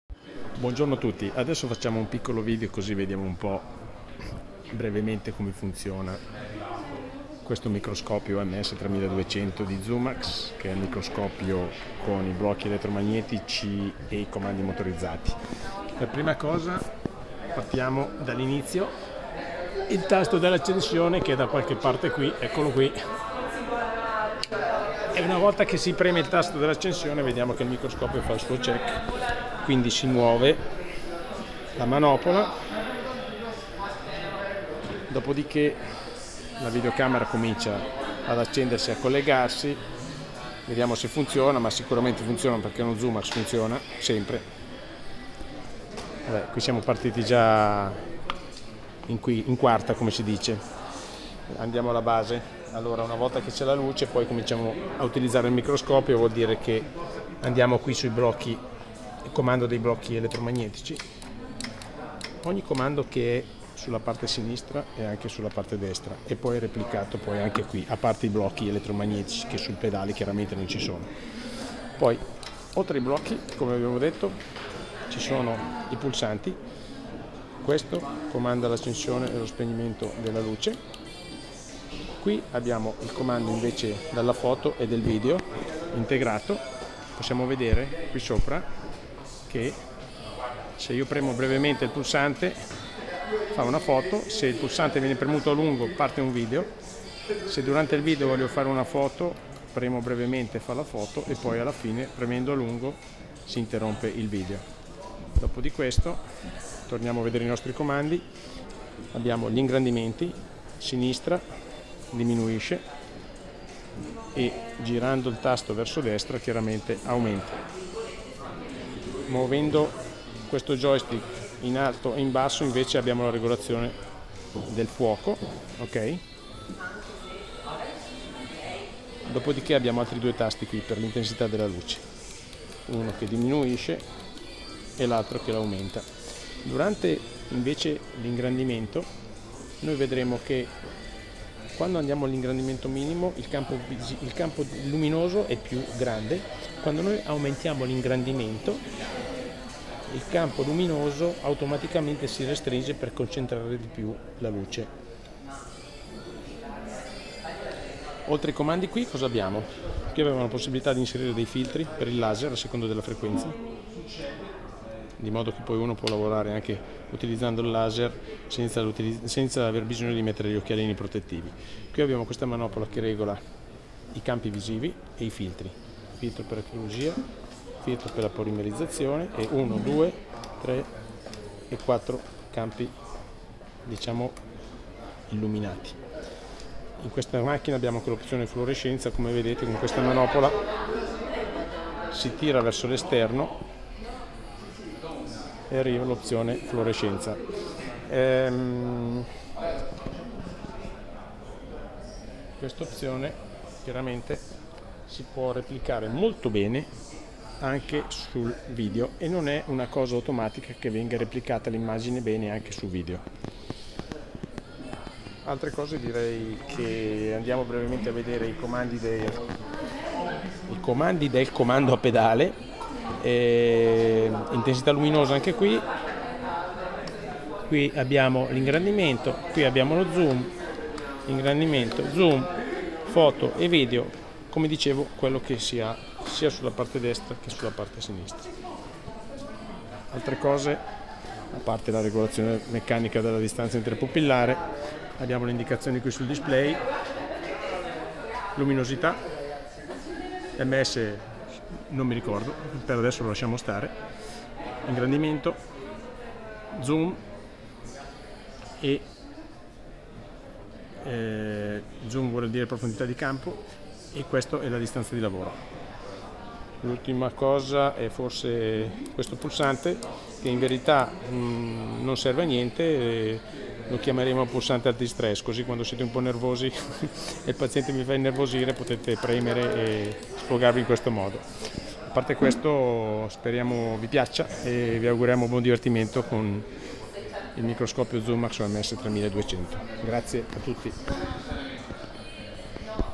OMS3200: Breve descrizione
Microscopio ZUMAX con blocchi elettromagnetici e comandi motorizzati